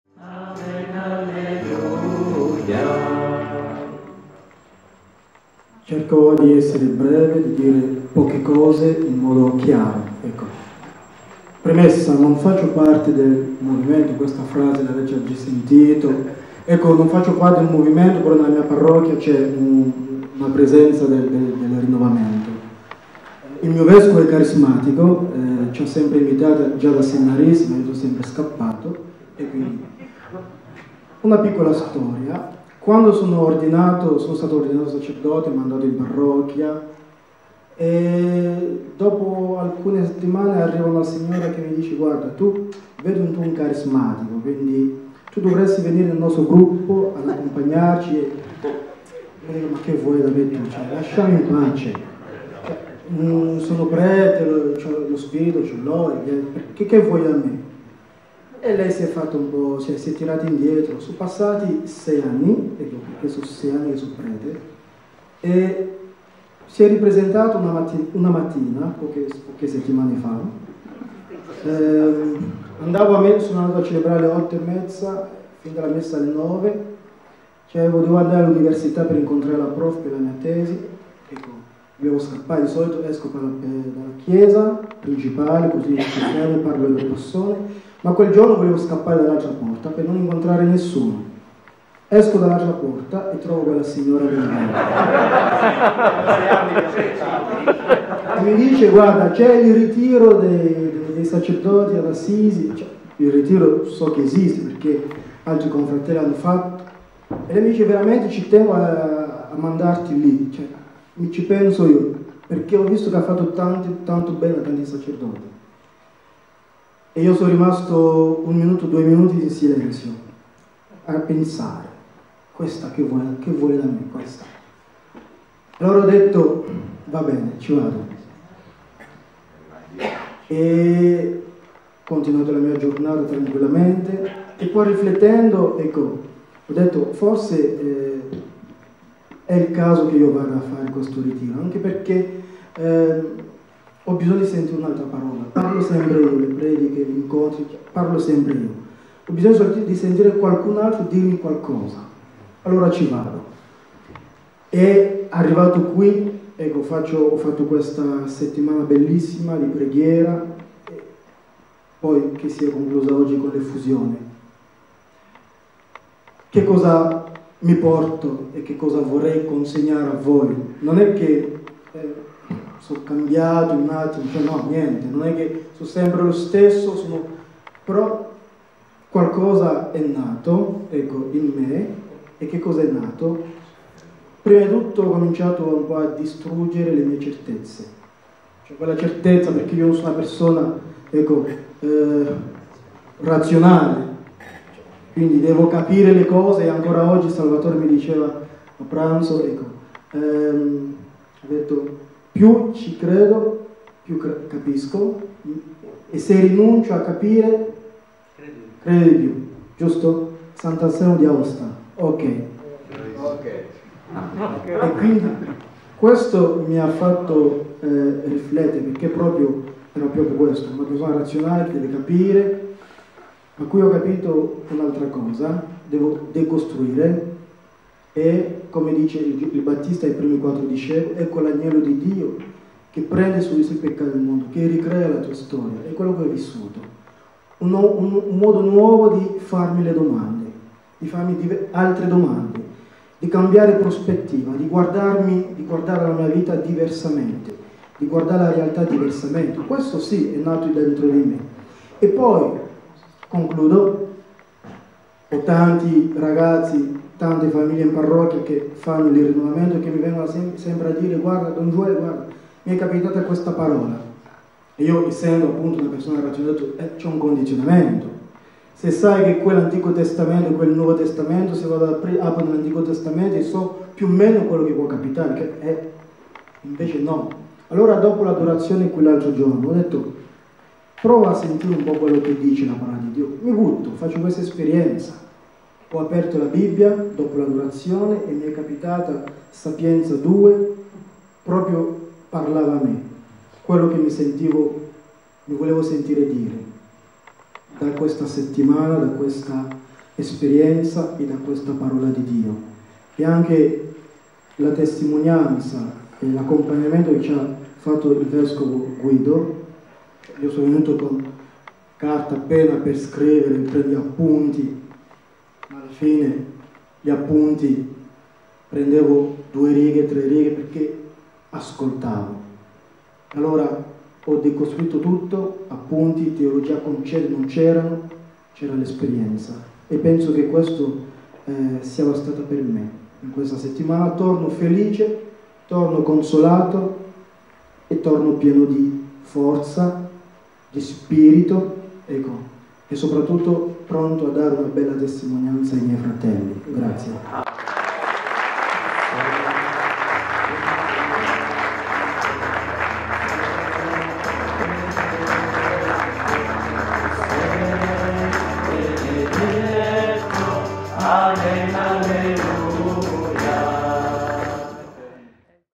TESTIMONIANZA SUL RITIRO NAZIONALE PER SACERDOTI, DIACONI E RELIGIOSI 2021 - Don Joel Nomo Atangana
Testimonianza video sul Ritiro Nazionale per Sacerdoti, Diaconi e Religiosi (Assisi, 8-13 novembre 2021) - Don Lazare Joel Nomo Atangana.
È possibile ordinare i VIDEO INTEGRALI del Ritiro Nazionale per sacerdoti, diaconi e religiosi 2021, cliccando sul seguente link: